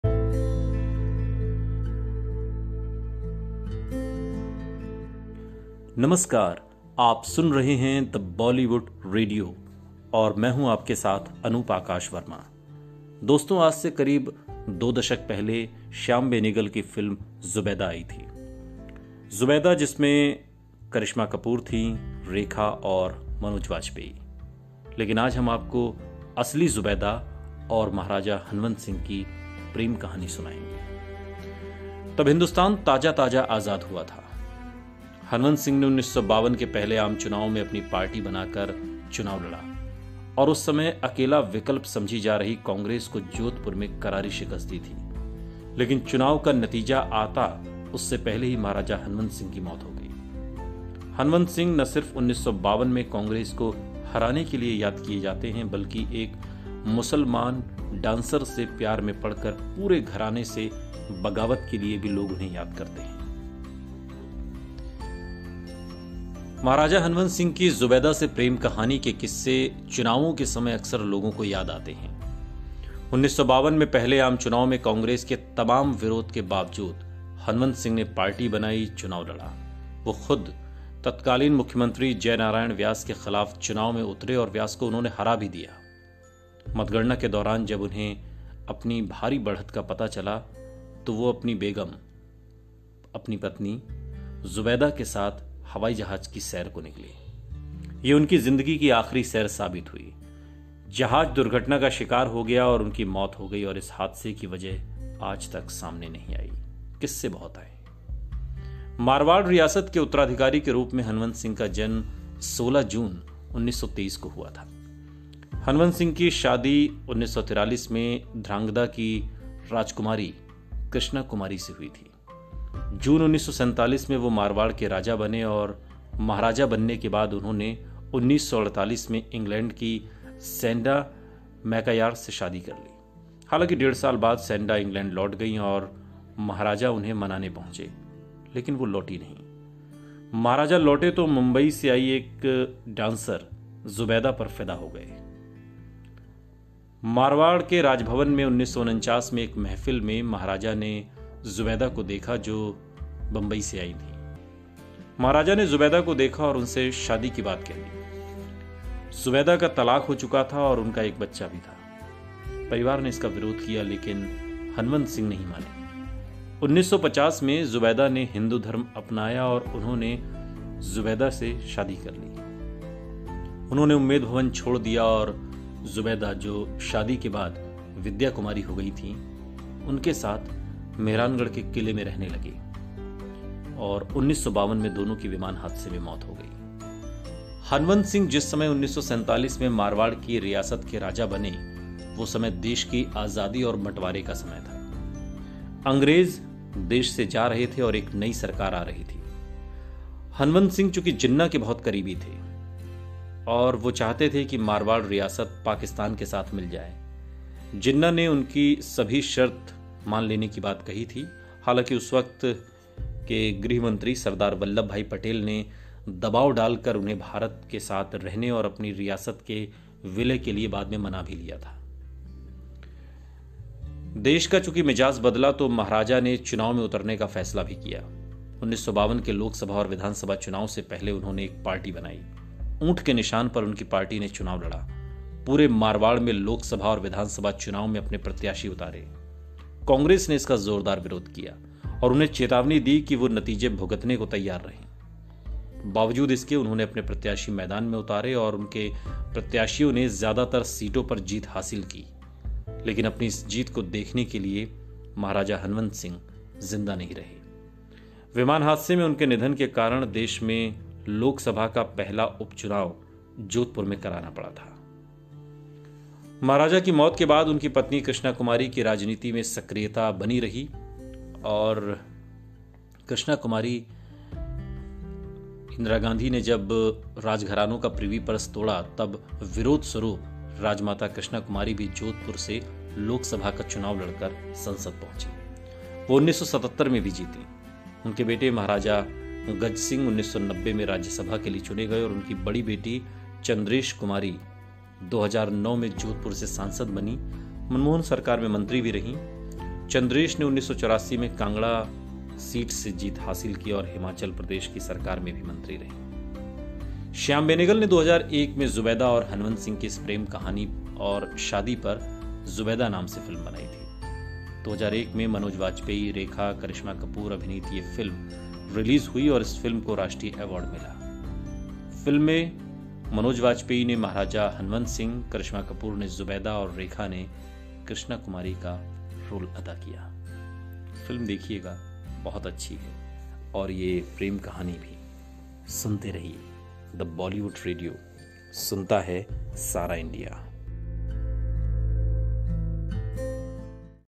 Shyam Benegal की फिल्म Zubeidaa की असली जुबैदा और Maharaja Hanwant Singh की कहानी
मारवाड़ राजभवन में 1949 में एक महफिल में महाराज ने जुबैदा (Zubeidaa) को देखा, जो मुंबई (Mumbai) से आईं थीं। महाराज ने जुबैदा को देखा और उनसे शादी की बात कह दी। जुबैदा का तलाक हो चुका था और उनका एक बच्चा था। परिवार ने इसका विरोध किया लेकिन हनवंत सिंह (Maharaja Hanwant Singh) नहीं माने। 1950 में जुबैदा ने हिन्दू धर्म अपनाया और उन्होंने जुबैदा से शादी कर ली। उन्होंने उम्मेद भवन छोड़ दिया और जुबैदा जो शादी के बाद विद्या कुमारी हो गईं थीं, के साथ मेहरानगढ़ के किले में रहने लगे। 1952 में दोनों की विमान हादसे में मौत हो गई, सुनिए पूरी कहानी का PODCAST अनूप आकाश वर्मा (Anoop Aakash Verma) के साथ